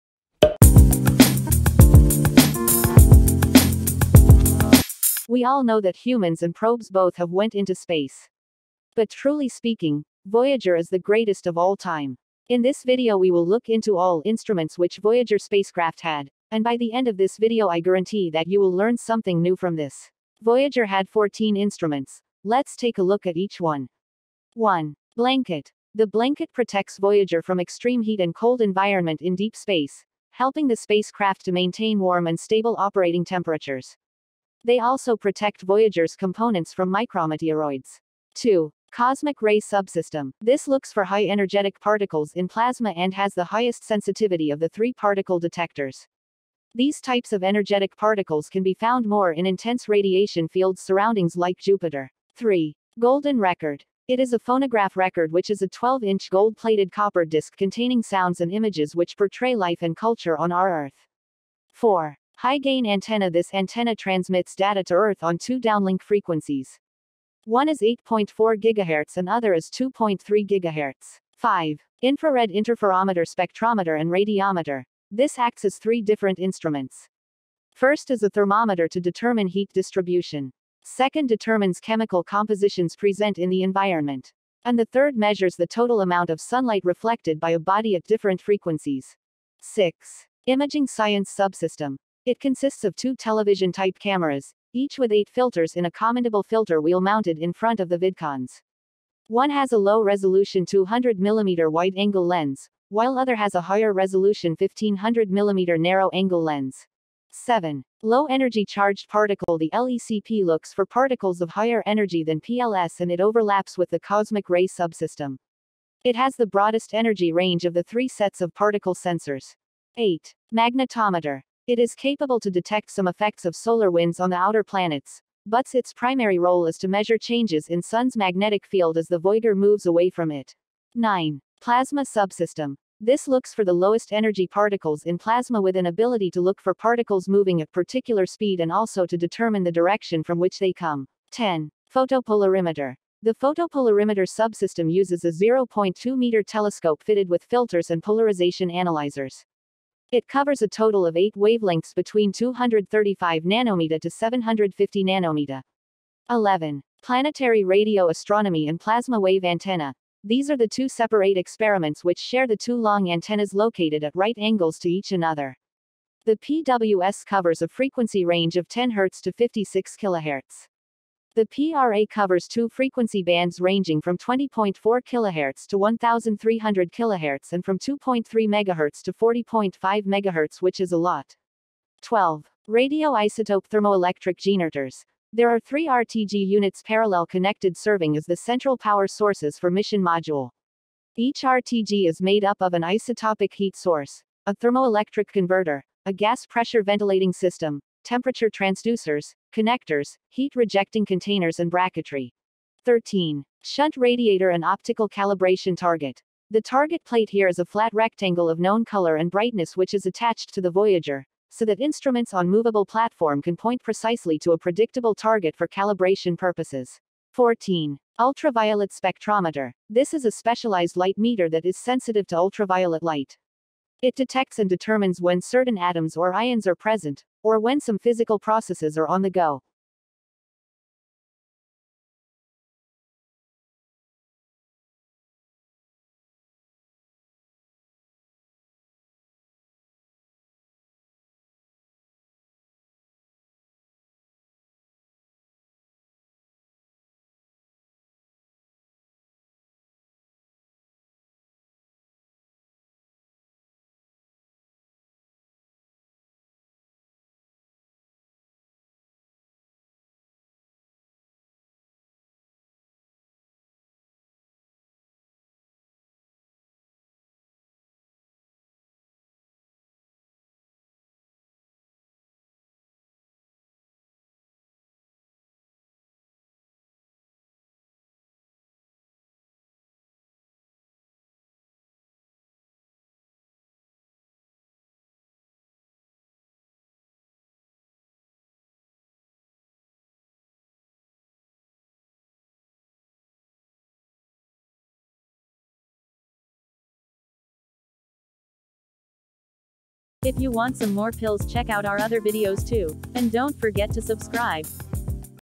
NASA's Voyager Mission Instruments Detail
Voyager had 14 instruments: Blanket, Cosmic Ray Subsystem, Golden Record, High Gain Antenna, Infrared Interferometer Spectrometer and Radiometer, Imaging Science SubSystem, Low-Energy Charged Particle, Magnetometer, Plasma Subsystem, Photopolarimeter, Planetary Radio Astronomy and Plasma Wave Antenna, Radioisotope Thermoelectric Genertors, Shunt Radiator and Optical Calibration Target & Ultraviolet Spectrometer.

The Voyager program is an American scientific program that employs two robotic spaceprobes, Voyager 1 and Voyager 2, launched in 1977 to take advantage of a favorable alignment of Jupiter, Saturn, Uranus, and Neptune. The Voyagers now explore the outer boundary of the heliosphere in interstellar space; their mission has been extended three times and they continue to transmit useful scientific data. Neither Uranus nor Neptune has had a close-up picture taken by a probe other than Voyager 2.

The Voyager spacecraft are the third and fourth human spacecraft to fly beyond all the planets in our solar system. Pioneers 10 and 11 preceded Voyager in outstripping the gravitational attraction of the Sun but on February 17, 1998, Voyager 1 passed Pioneer 10 to become the most distant human-made object in space.